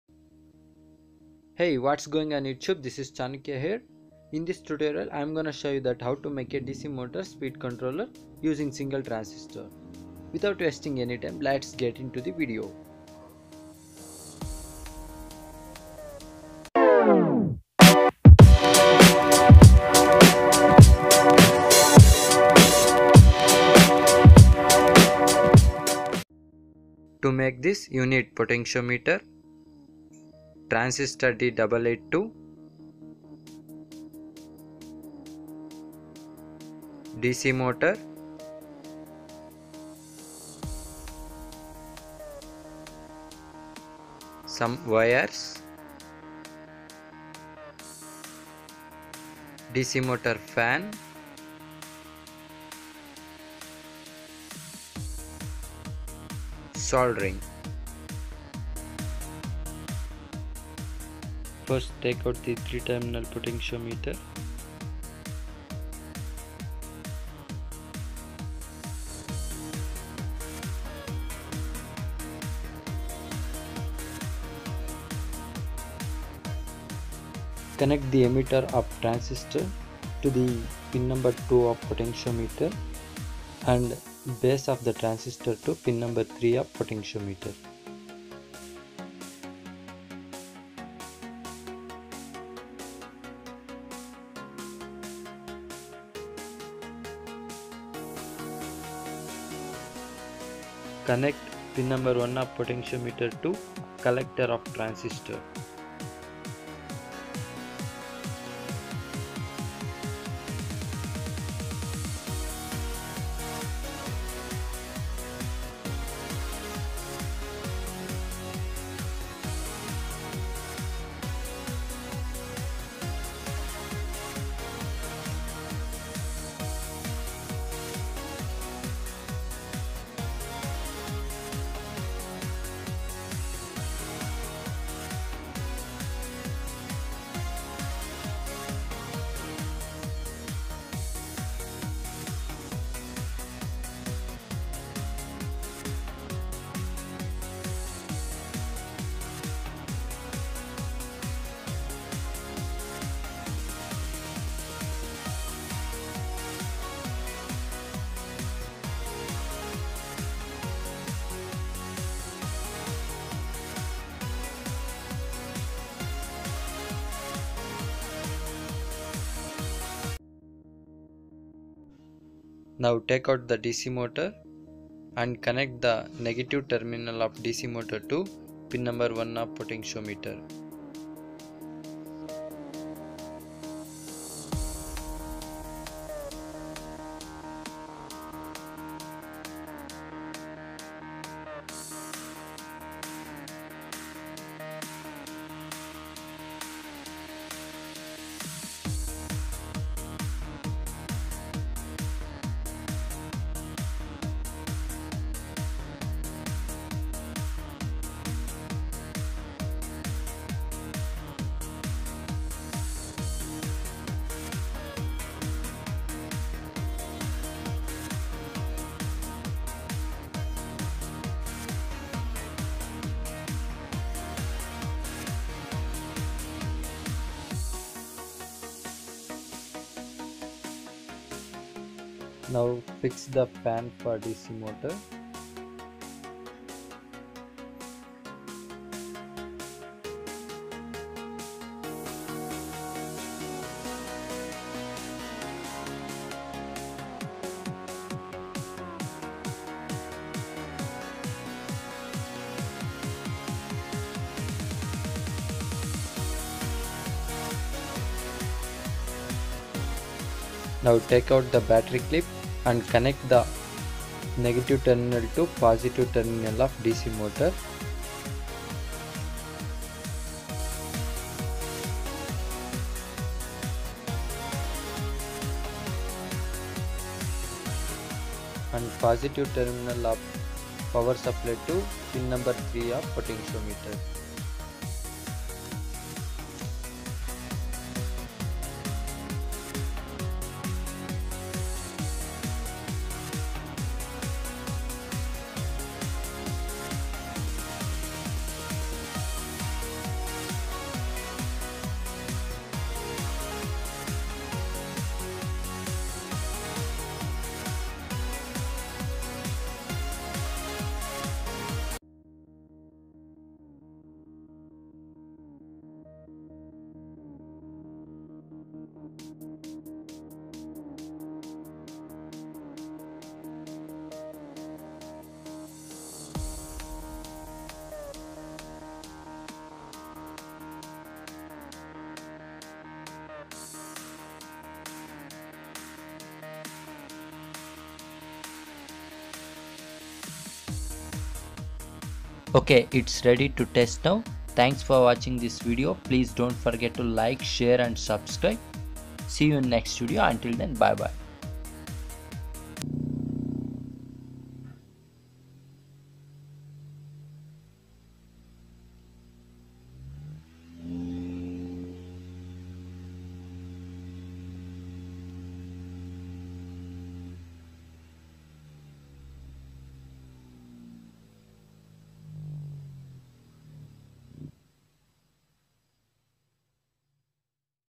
How to make simple DC motor speed controlling fan
DC motor speed controller using transistor D882

Components required:

1.dc motor
2.transistor D882
3. potentiometer
4. Wires
5. Motor fan (propeller)
6. Battery
7. Battery clip